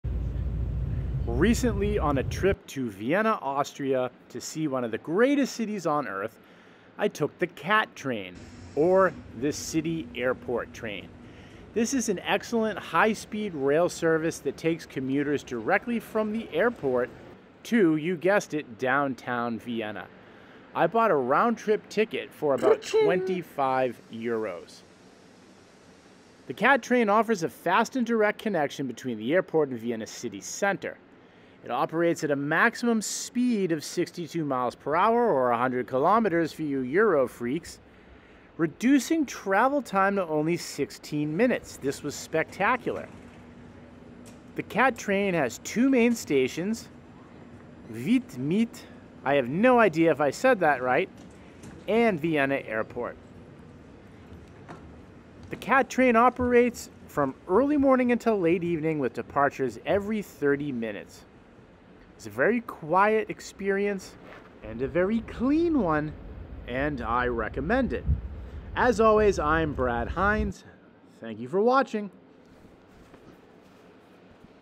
[Travel Austria] Riding The CAT Train Airport Express To Downtown Vienna From Vienna Airport
[Travel Austria] How To Use The CAT train To Go Express To Downtown Vienna From Vienna Airport

Here are some key features and details about the CAT train:

Speed and Frequency: The CAT train offers a fast and direct connection between the airport and Vienna city center. It operates at a maximum speed of 100 km/h (62 mph), reducing travel time to approximately 16 minutes.

Stations: The CAT train has two main stations: Wien Mitte and Vienna Airport. The Wien Mitte station is located in the city center, close to popular attractions, shopping centers, and major transportation hubs. The Vienna Airport station is situated directly at the airport terminal, providing convenient access to both the arrivals and departures areas.

Schedule and Frequency: The CAT train operates from early morning until late evening, with departures every 30 minutes. The exact timetable may vary, so it's advisable to check the official CAT website or consult the displayed schedules at the stations.